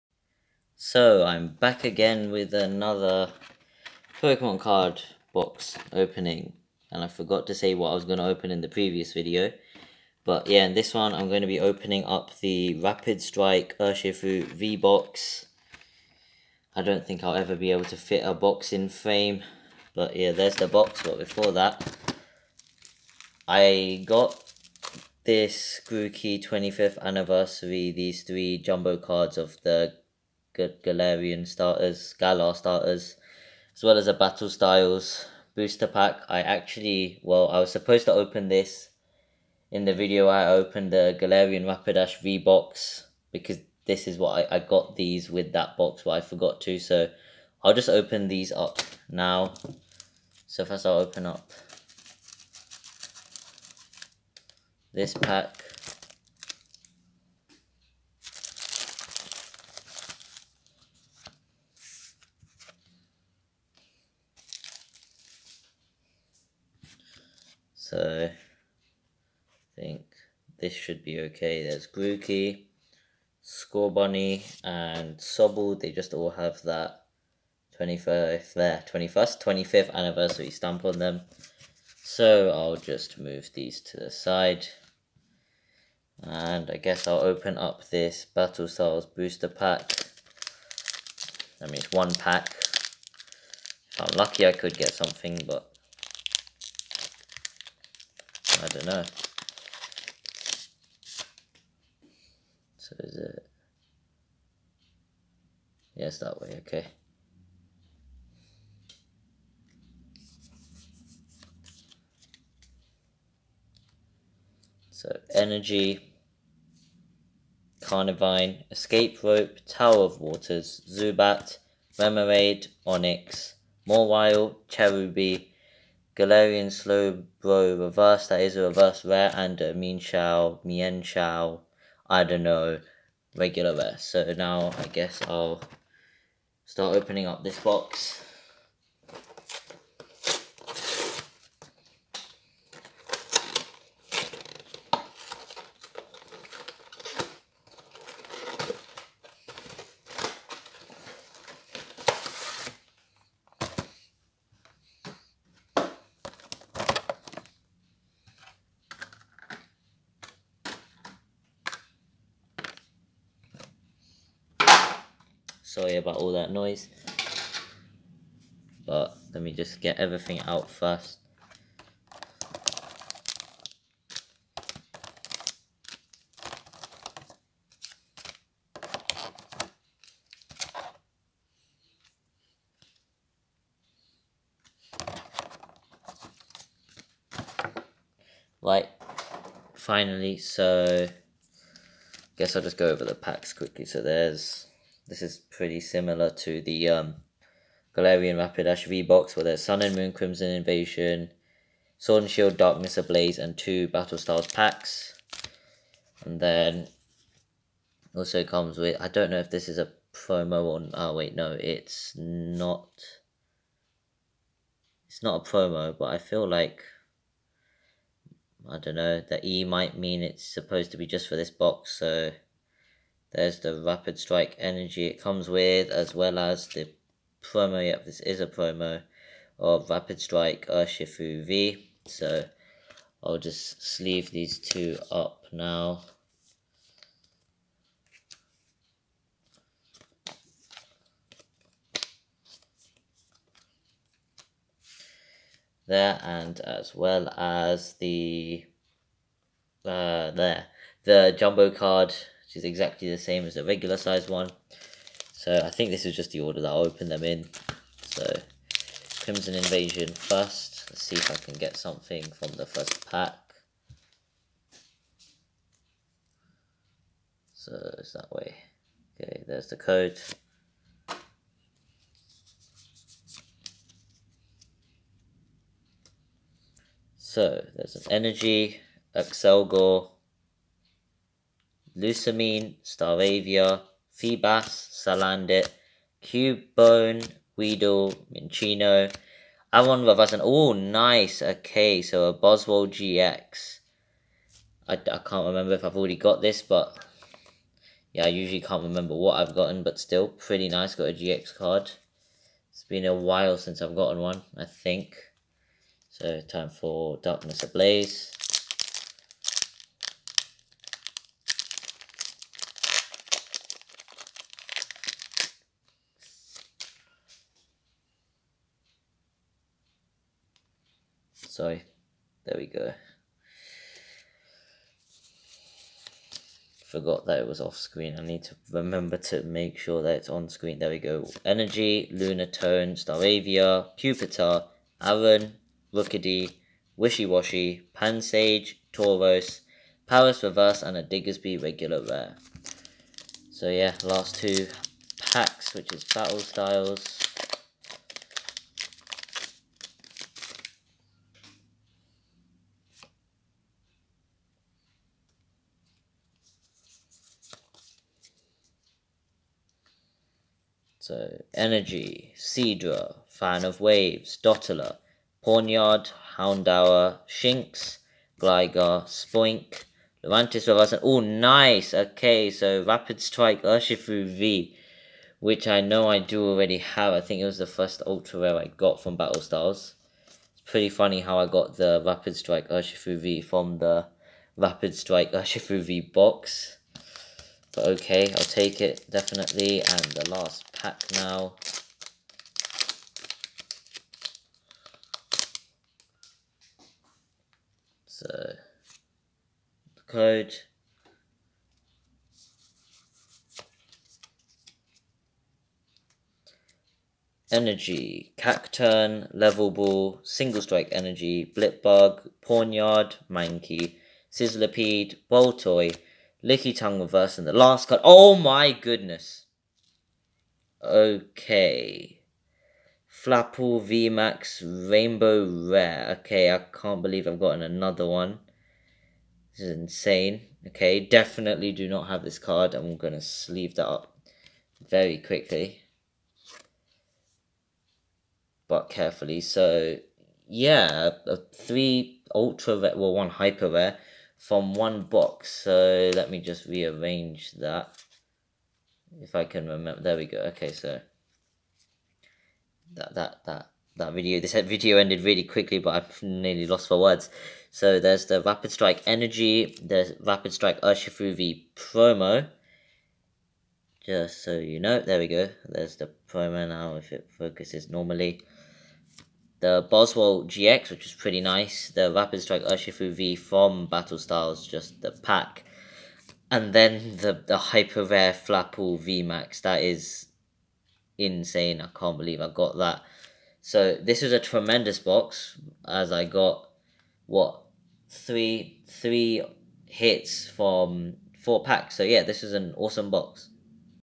Pokemon Rapid Strike Urshifu V Box Opening
I open a Pokemon Rapid Strike Urshifu V Box in this video and get some amazing cards! Watch the video to find out what I get!